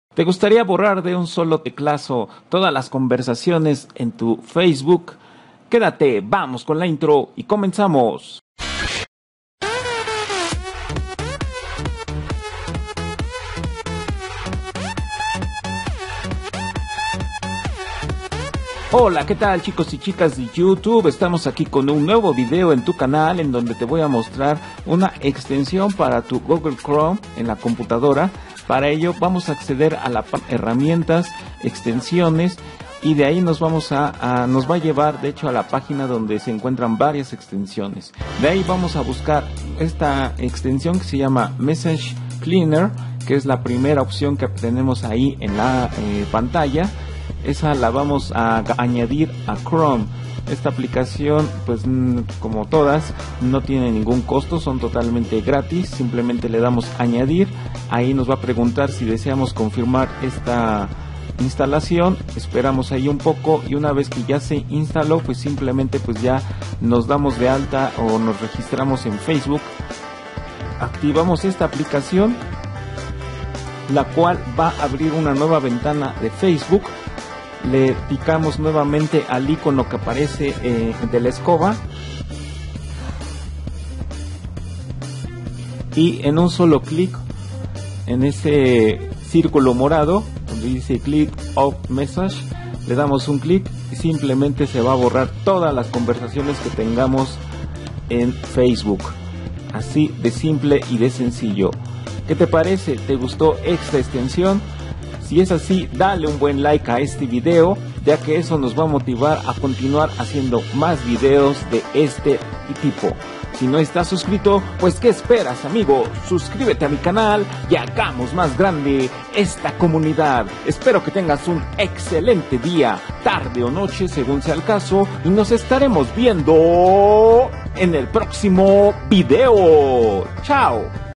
Como eliminar todos los mensajes de facebook en un solo clic
te muestro como eliminar todos los mensajaes que tengas en tu facebook de un solo click,

Hasta pronto. :)

See ya